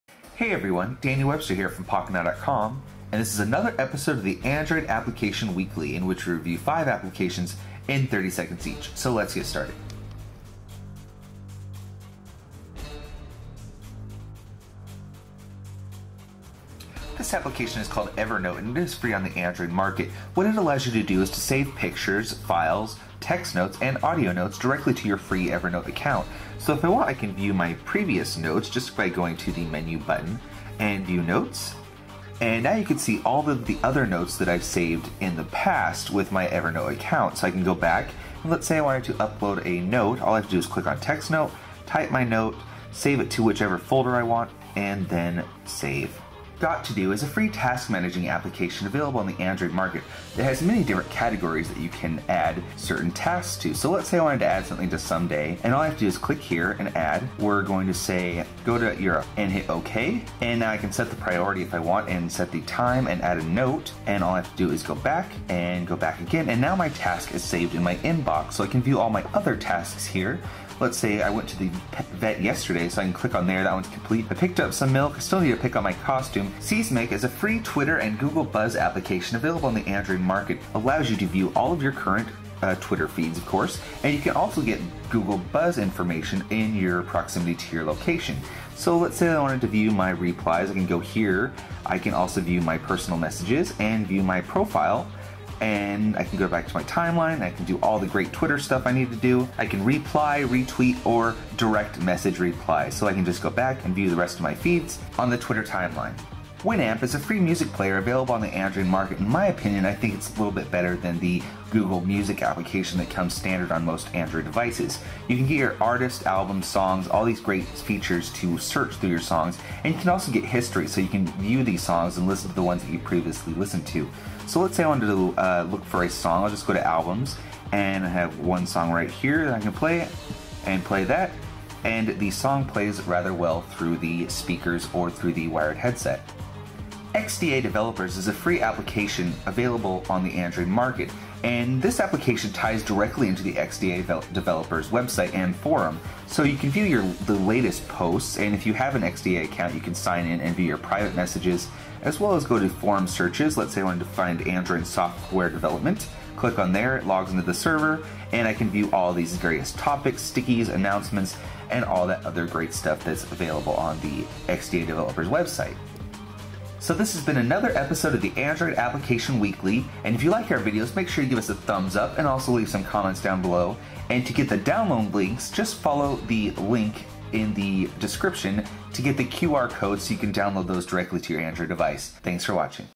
Android Application Weekly 29 Oct 2010 | Pocketnow
For the QR-Codes To conclude the final week of October we have five useful applications in this episode of the Android Application Weekly. In this segment we review five applications in 30 seconds each. In this episode we have; Evernote, Got To Do, Seesmic, Winamp, and xda-developers.